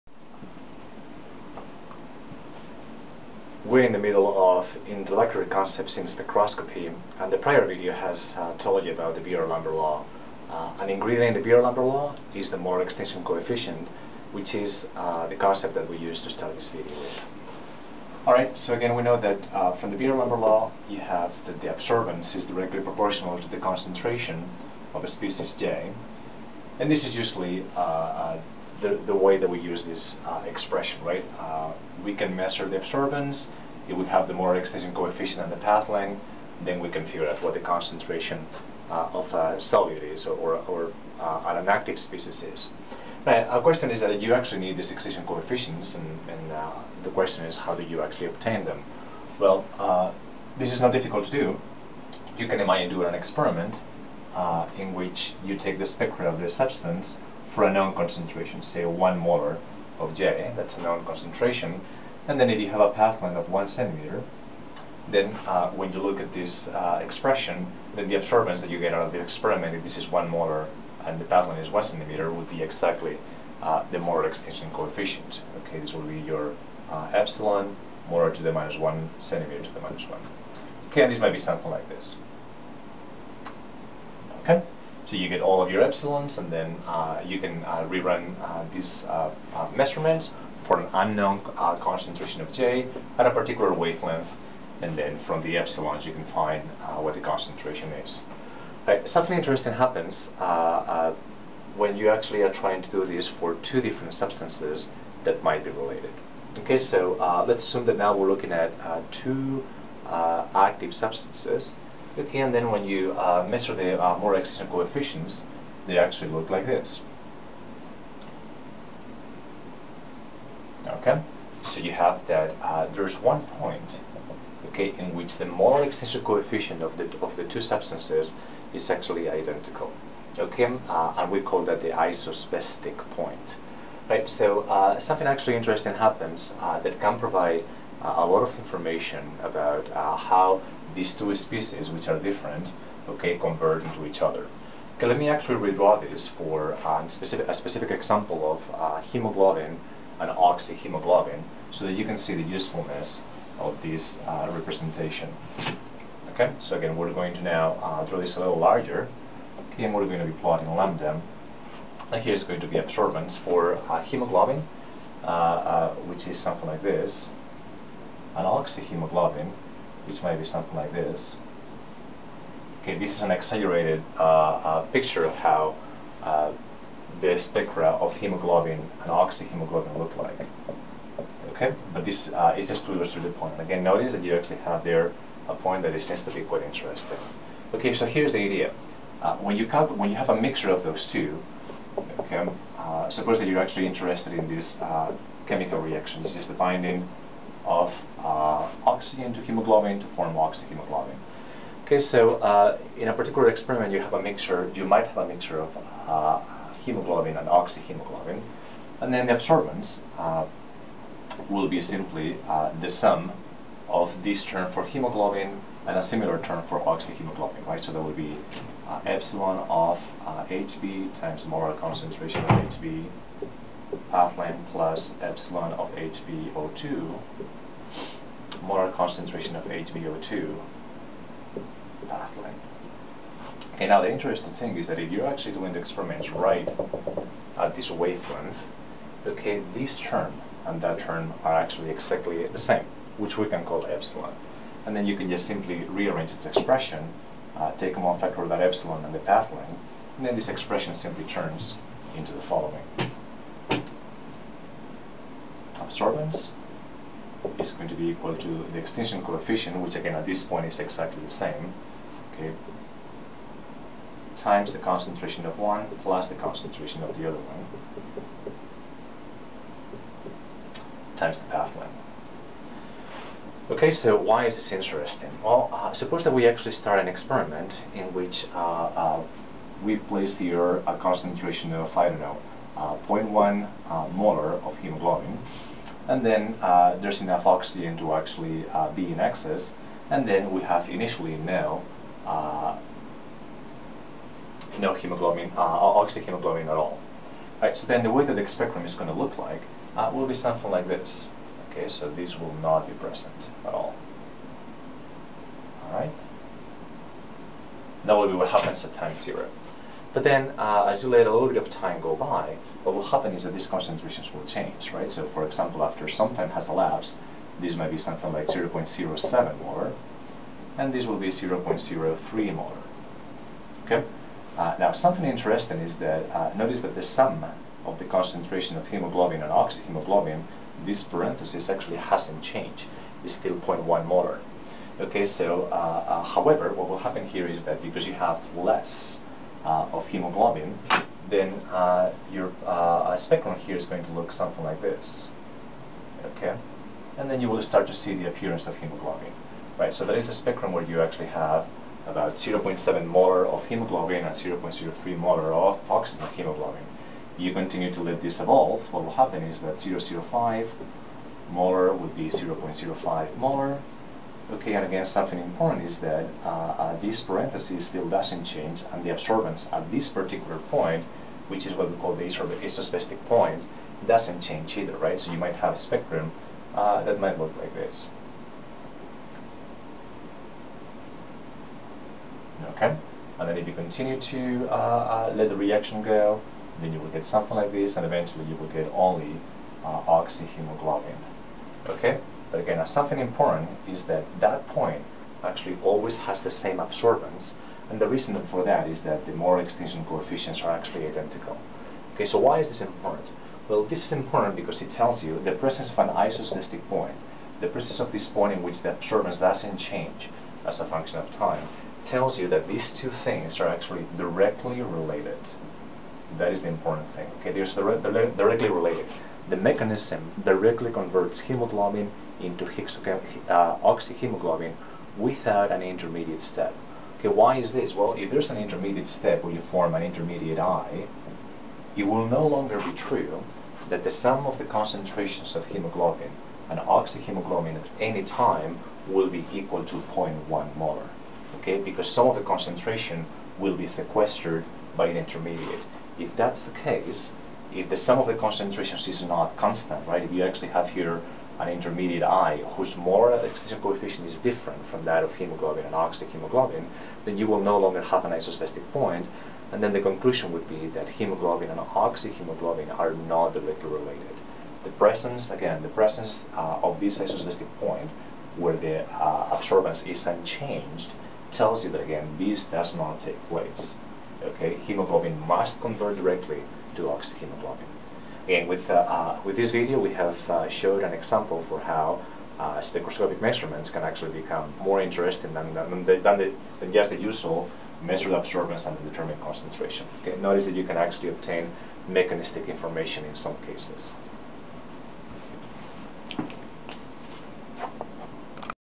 Spectroscopy. The isosbestic point
14-4. This video introduces the determination of the molar absorptivity and describes the isosbestic point and its mechanistic importance.